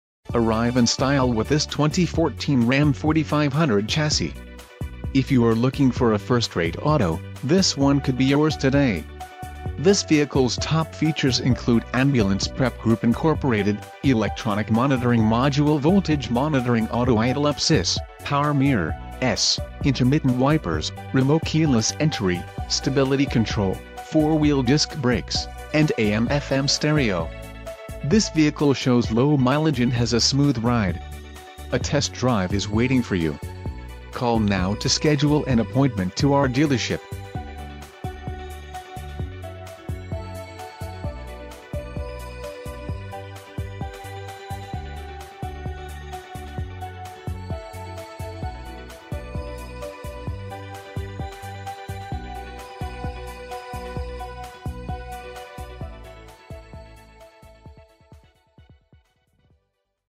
2014 Ram 4500 - Benny Boyd CDJ - Lampasas, TX 76550
4 Wheel Drive!!!4X4!!!4WD. All the right ingredients! This fabulous Vehicle will have you excited to drive to work, even on Mondays** Just Arrived.. Oh, and did you notice that it's generously equipped with options: Engine: 6.7L I6 Cummins Turbo Diesel, Transmission: 6-Speed Auto Aisin AS69RC HD, Radio: Uconnect 5.0 AM/FM/BT, Ambulance Prep Group, Full Size Spare Tire, Trailer Brake Control, Remote Keyless Entry, Chrome Appearance Group, DOT Certified Roadside Safety Kit... A Better Buy from Benny Boyd!